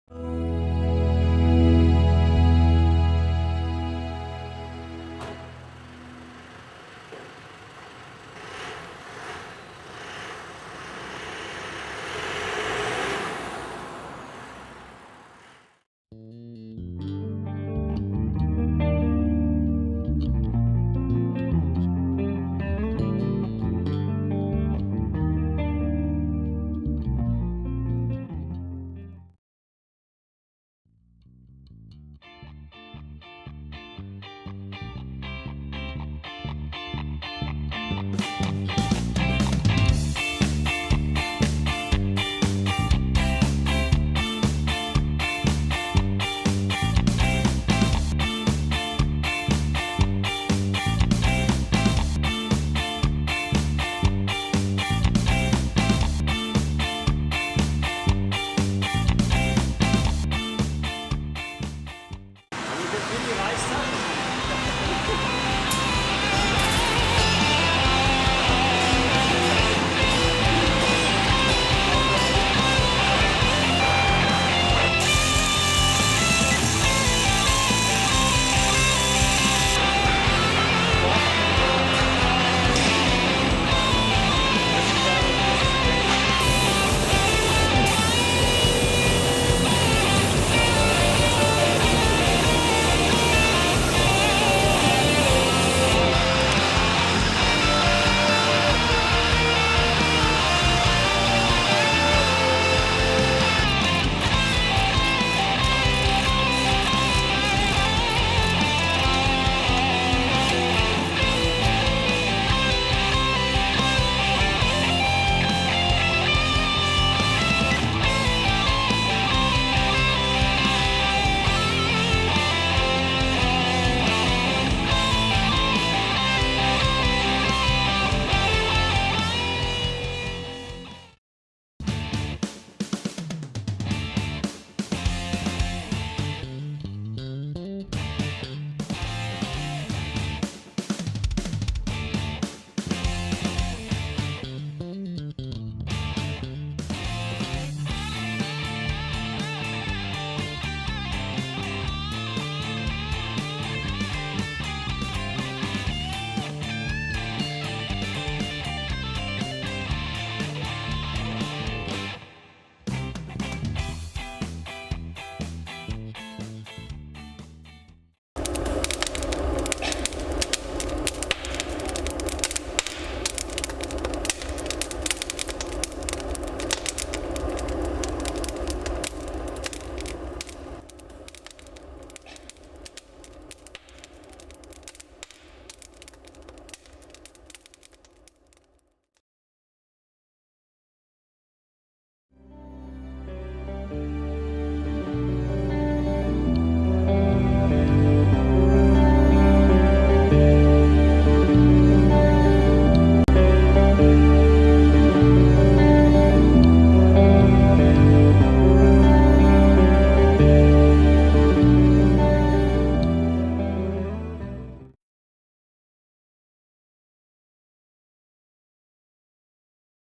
HERZ firestar Holzvergaserkessel
Projekt mit der HBLA für Produktmanagement und Präsentation - Oberwart